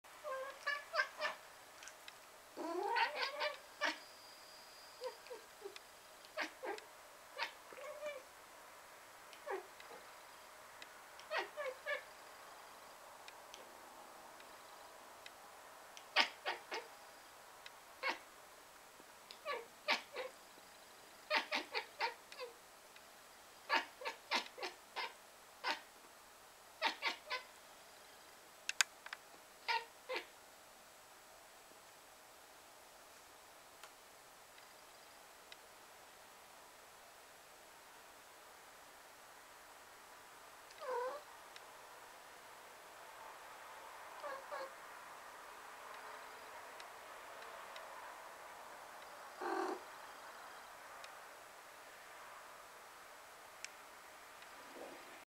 Cat disgusted
Mambo obserwował przez okno latające ptaki. Miał rozrywkę. Gdy zniknęły z pola widzenia ... efekt widać na końcu filmu.

Mambo watched flying birds through the window. He had fun. When they disappeared from the field of view... the effect can be seen at the end of the film.